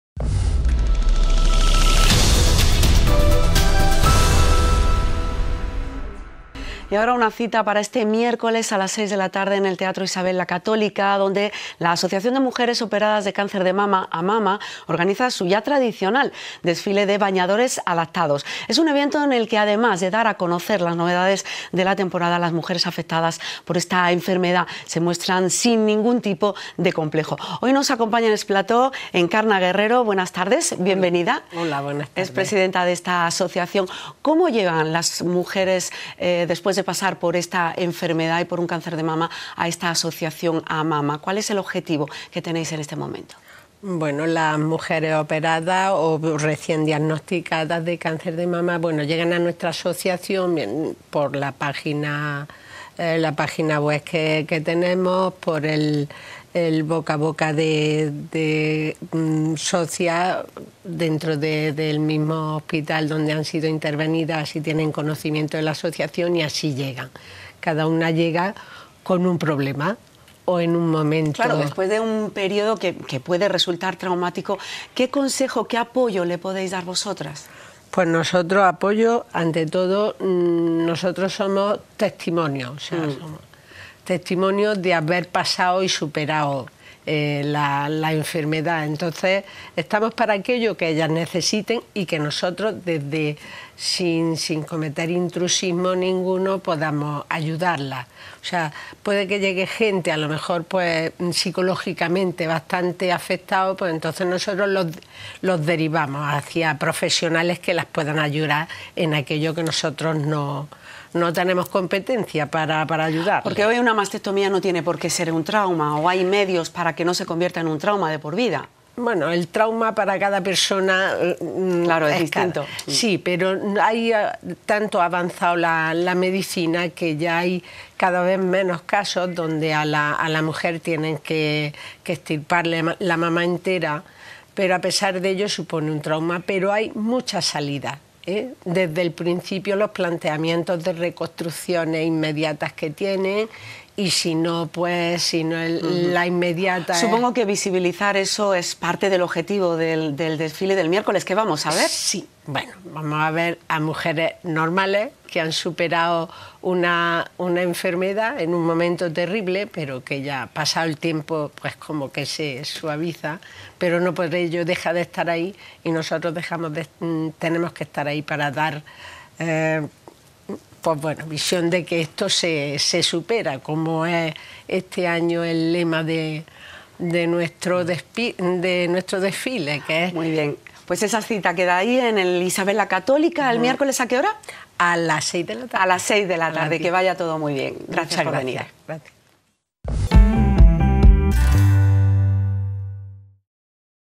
Entrevista desfile de bañadores AMAMA
Desfile de bañadores adaptados para operadas de cáncer de mama.